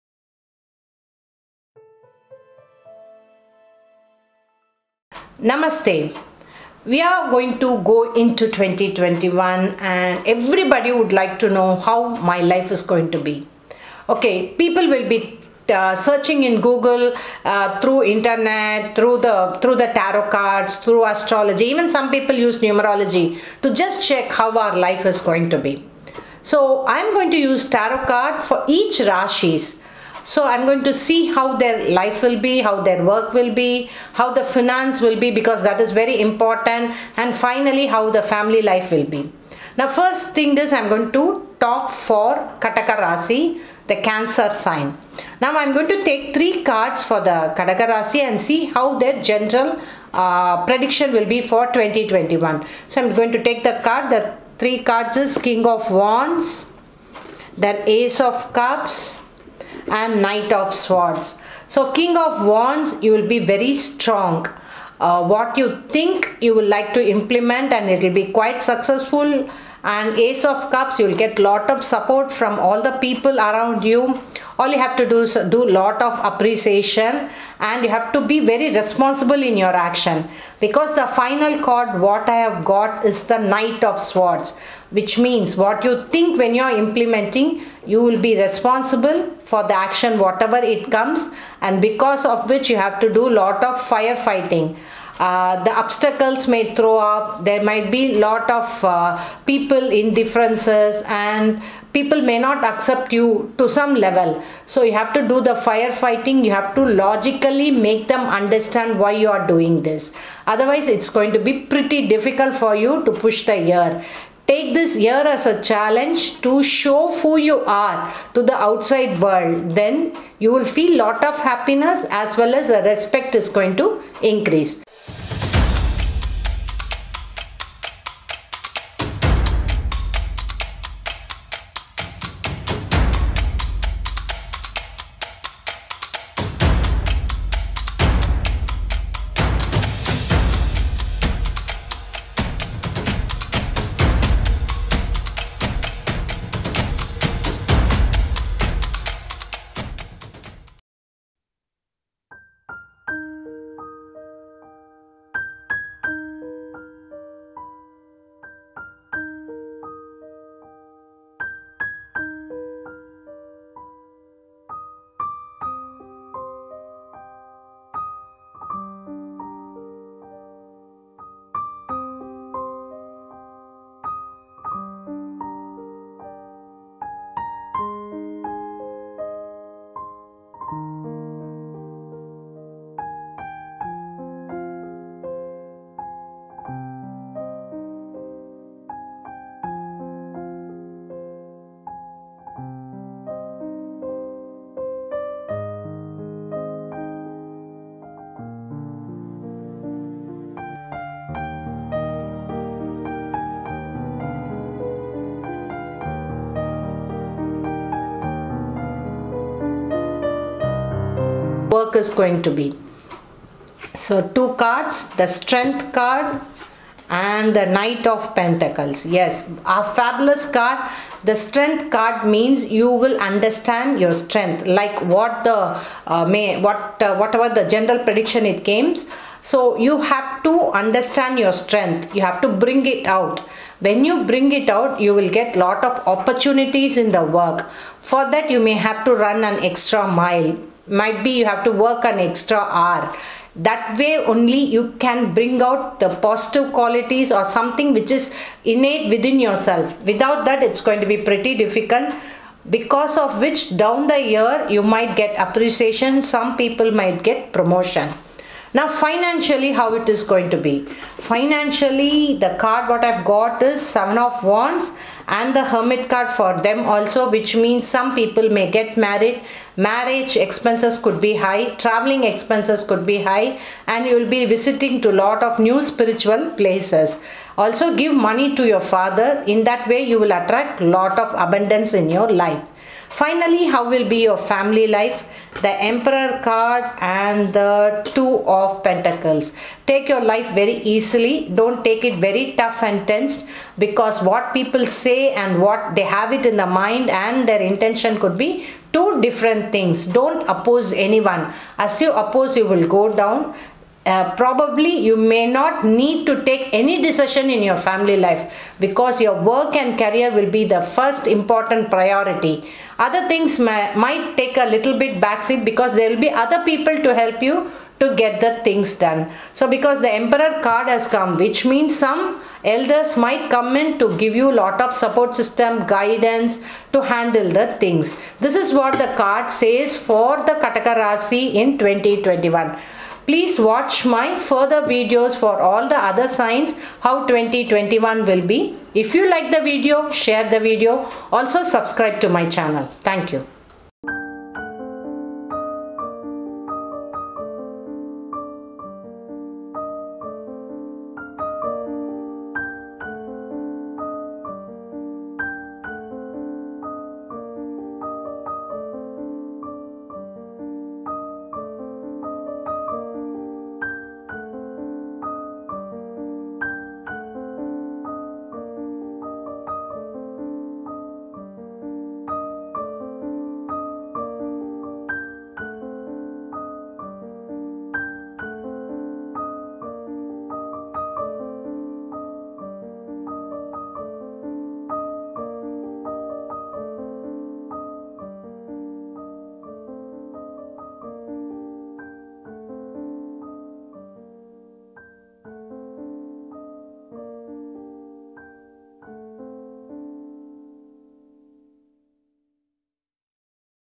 How will be 2021 for Kadaka Rashi ? TAROT READER JAYASHREE ON CAREER AND FINANCE
FOR CONSULTATION  :
HOMAM AGAINST COVID - 19 and OMICRON